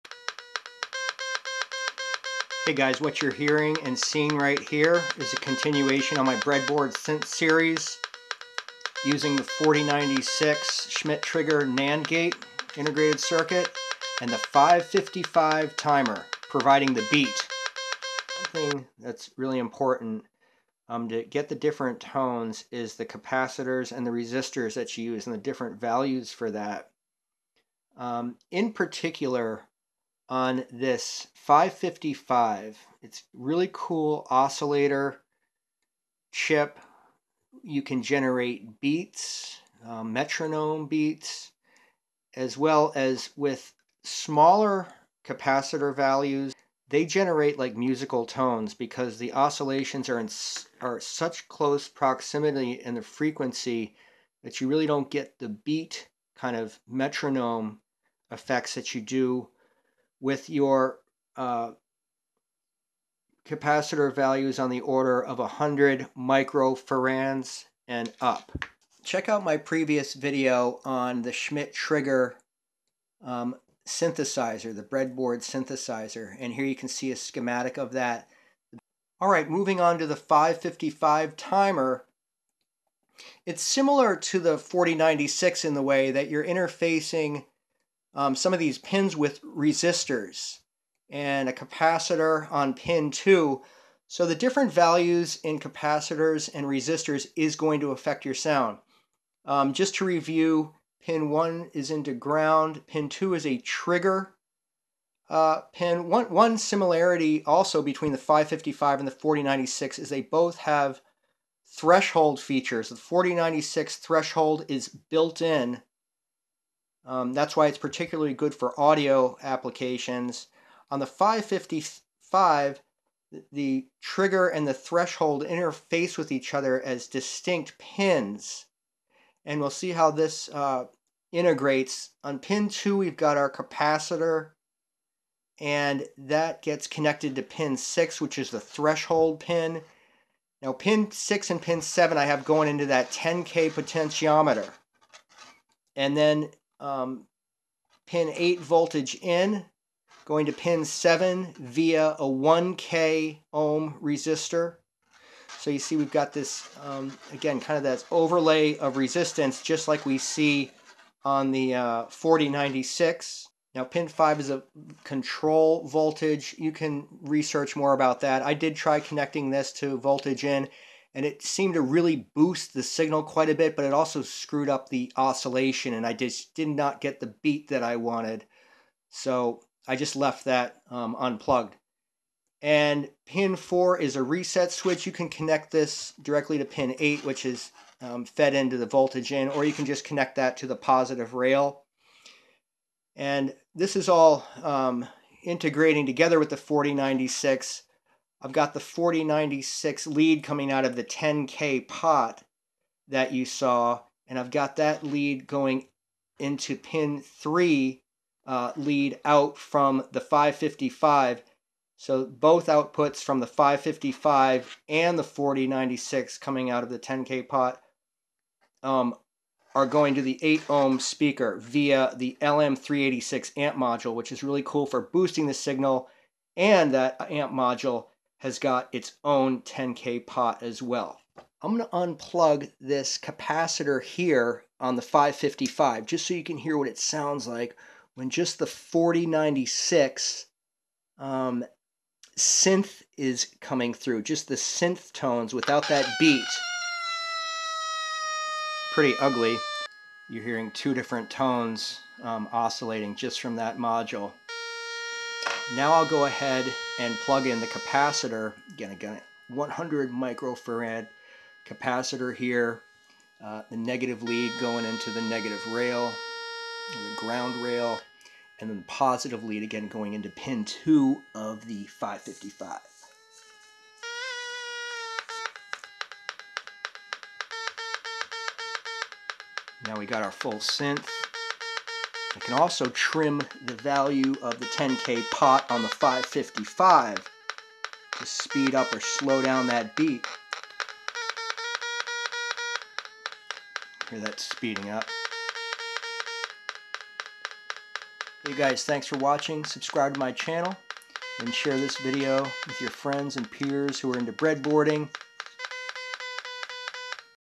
DIY analog synth for $5
Learn integrated circuit fun by making oscillating synthesizer. 4093B Schmitt Trigger NAND logic gate provides musical tones, while 555 timer chip generates beats. Pause video to review schematics.